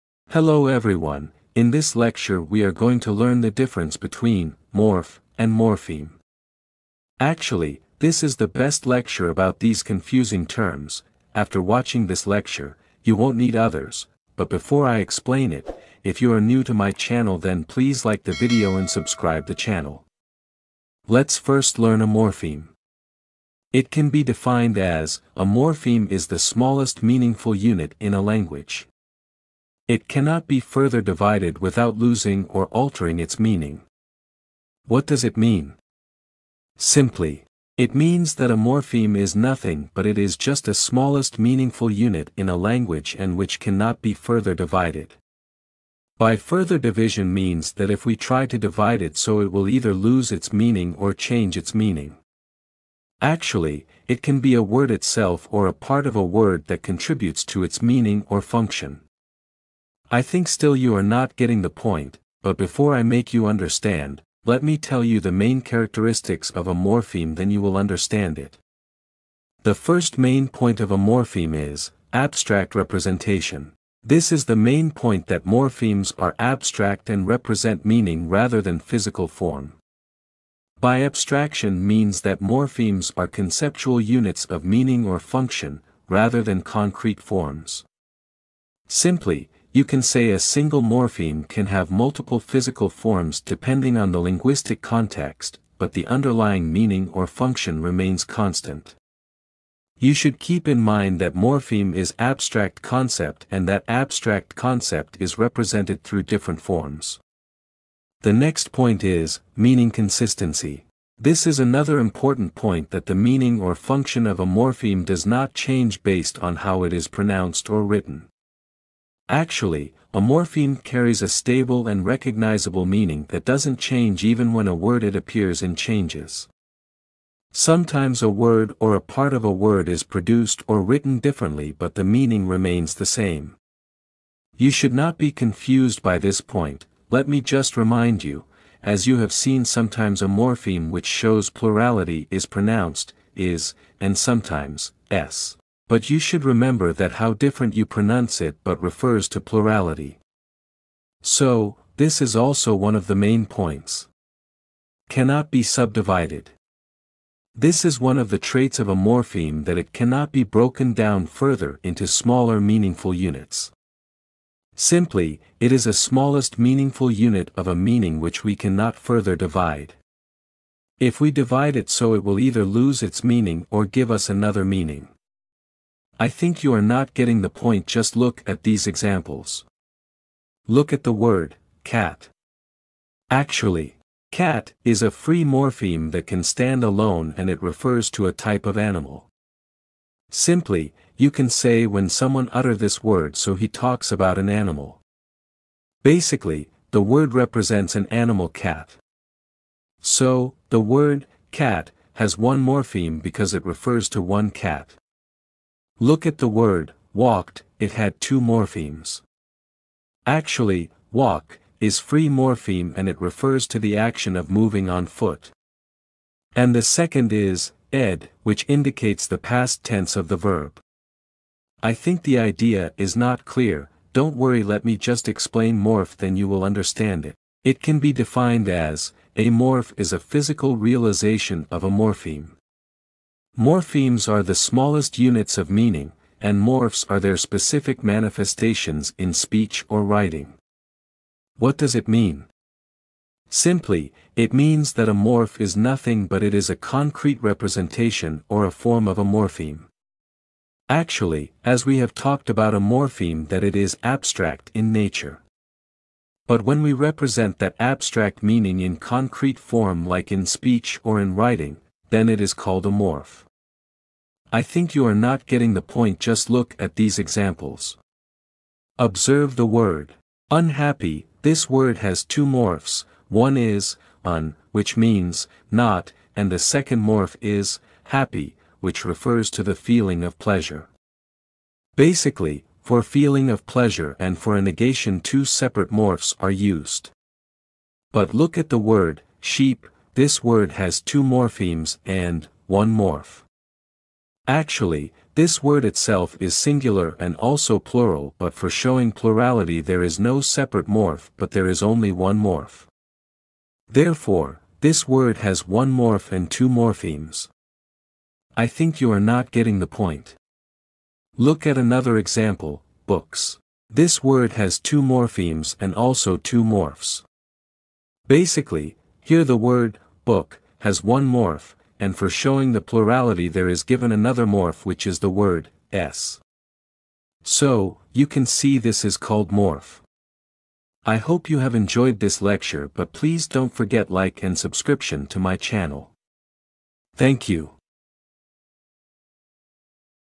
Morph and morpheme | Morph vs morpheme | What is morphe?
in this lecture the difference between morph and morpheme is explained in detail with numerous examples. Watch it till end and learn the difference.